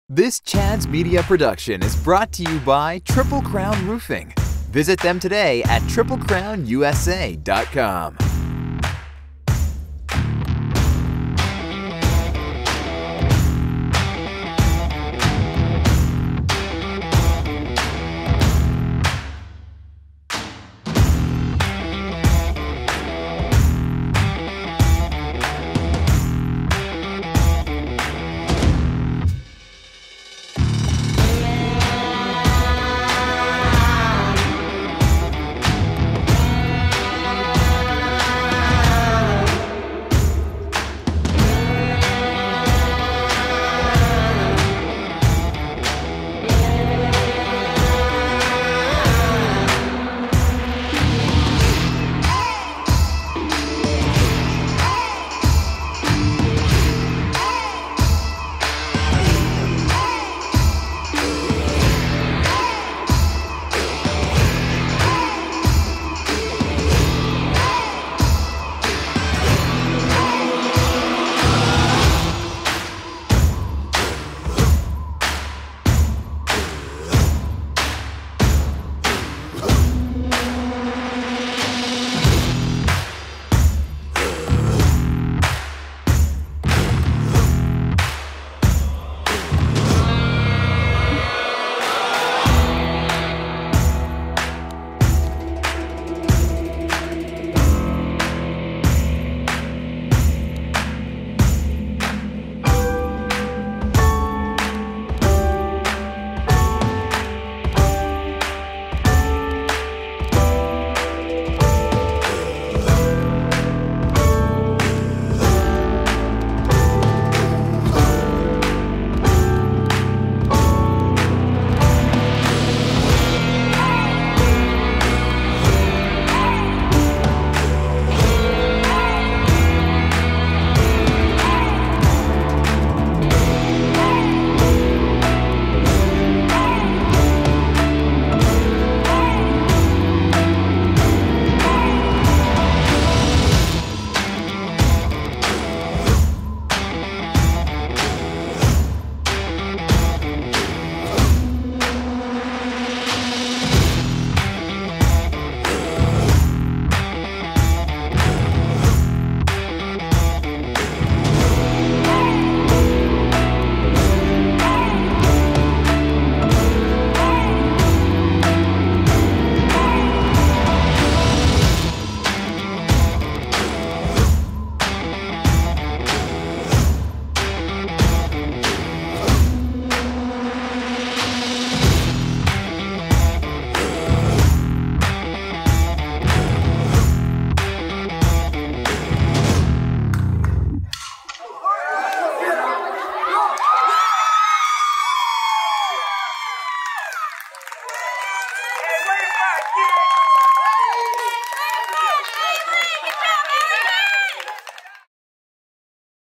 LADYCATS SOFTBALL 4-19-19
The Sulphur Springs High School Lady Cats softball team traveled to Texarkana last night to put the final nail in their district championship run. AND THEY DROVE IT HOME. Your LadyCats are the sole 15 5-A District Champs and are headed to the playoffs. Big thanks to Triple Crown Roofing for their support of all thing Sulphur Springs, visit them or follow them on FB as a Thank You.